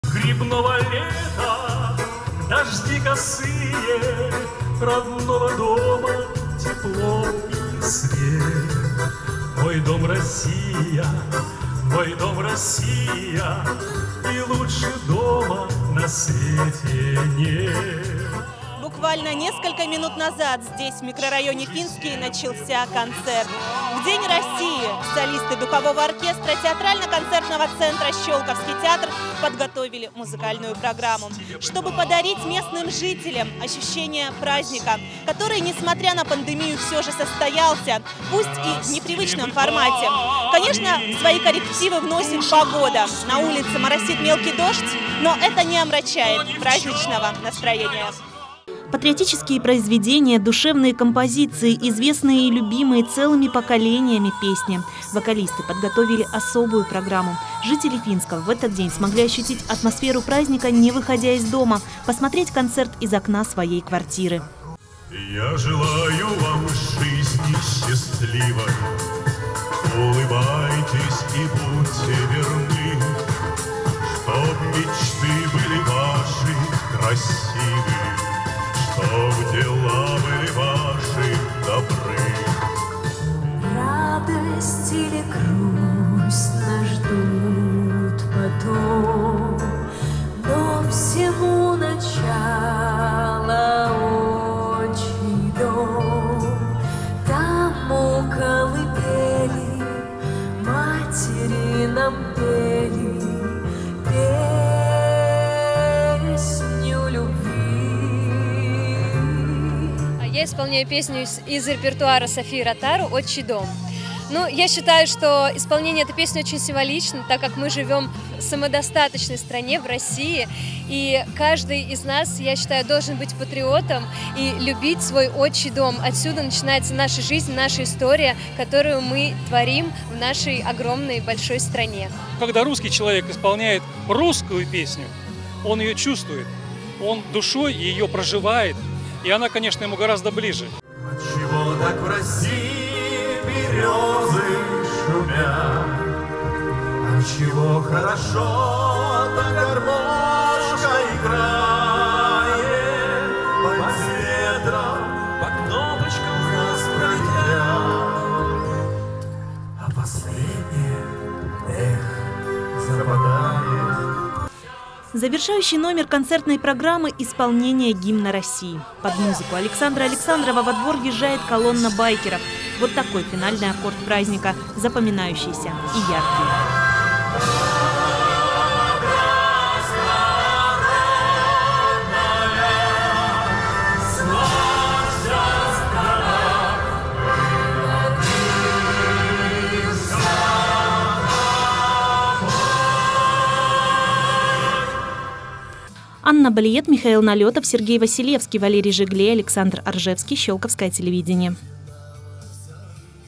День России – в сердце каждого. Праздничный концерт в мкрн.Финский
Праздничные концерты, приуроченные 30-летию принятия Декларации о государственном суверенитете страны, прошли в городском округе. О том как проходили мероприятия в мкрн.Финский наш репортаж